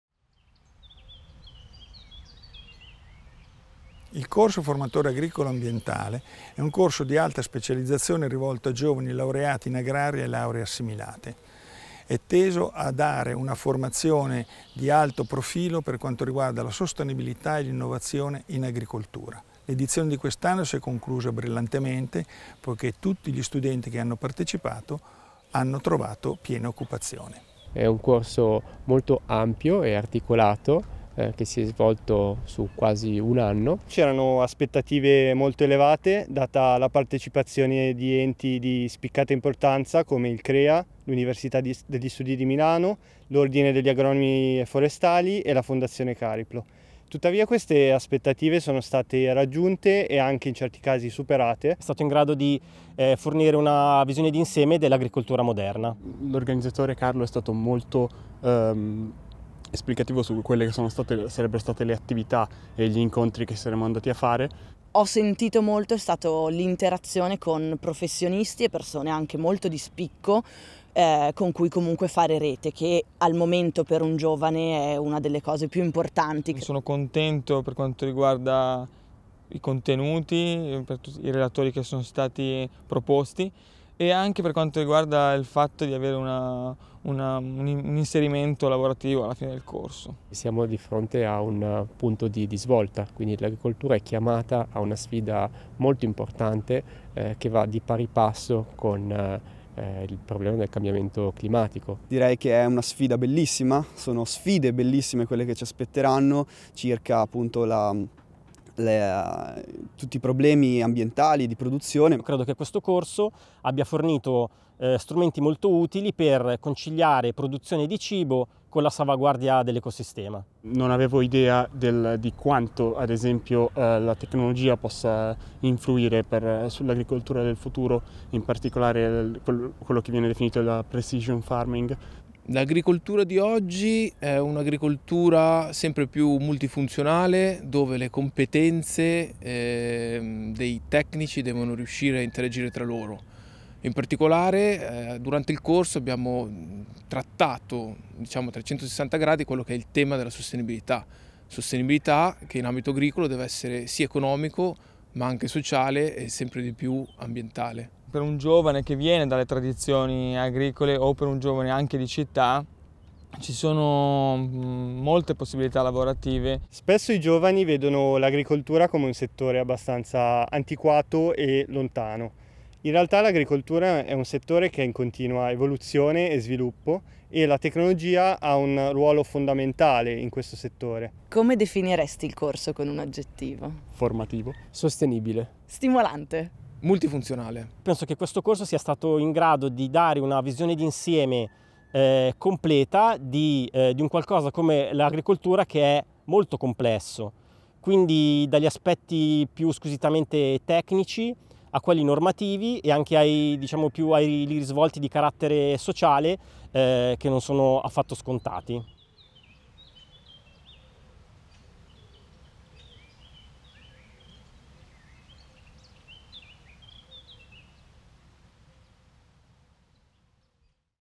Master FAA
Il Corso del Formatore Agricolo Ambientale raccontato dai protagonisti.